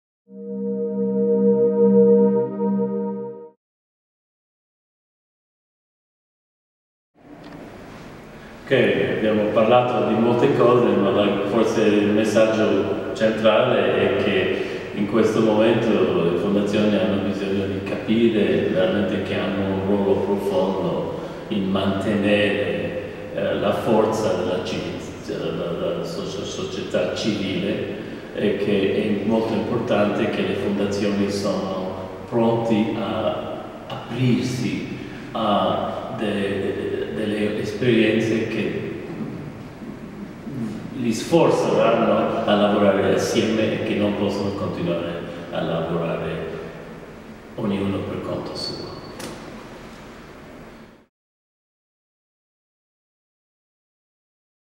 Gerry Salole, Convegno Nazionale Assifero, Padova 15-16 maggio 2019
Il messaggio rivolto alle fondazioni italiane di Gerry Salole, CEO del European Foundation Centre durante il Convegno Nazionale Assifero 2019